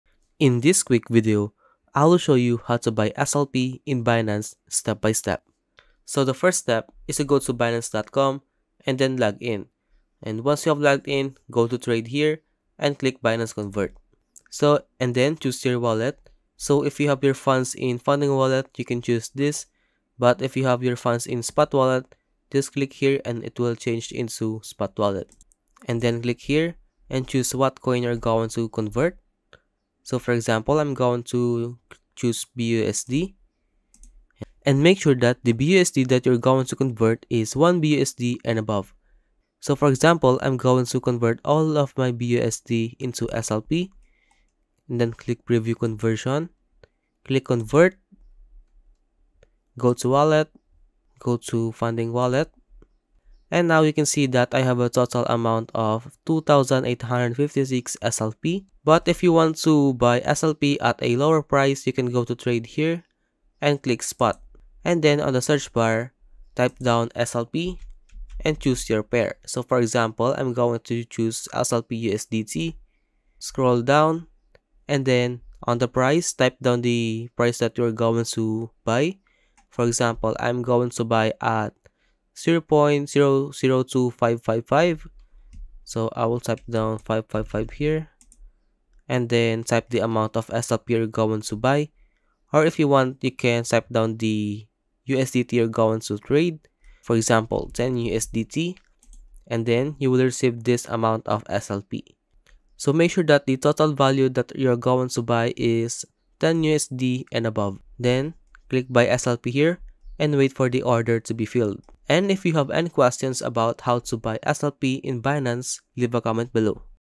How to Buy SLP in Binance (Step by Step)
In this quick video, I will show you How to Buy SLP in Binance step by step.

"Are you excited to start your journey towards earning some SLP with Axie Infinity? Do you want to know how to buy SLP in Binance?"

"Firstly, you'll need an account with Binance, so if you don't have one yet, make sure to sign up and get verified first. Once you're all set up, it's time to buy some SLP."

"To buy SLP on Binance, go to the Binance trade screen and search for SLP. Once you locate SLP, select it to view the SLP to Binance option."

"Simply enter the amount of SLP you want to buy and confirm the transaction. It's that easy!"

"But what about transferring your SLP to and from Binance? Transfer your SLP to Binance by selecting the 'Deposit' option, find your SLP transfer address, and use that to transfer SLP from your personal wallet."

"If you need to transfer SLP from Binance to your Ronin wallet, get the Ronin wallet address and put that in the 'Withdraw' option in Binance."

"Easy as pie! And if you ever need to buy SLP using Binance and Gcash, you can simply use your Gcash mobile wallet to fund your Binance account and buy SLP."

"And there you have it! Now you know how to buy, transfer, and withdraw SLP to and from Binance. Happy trading!"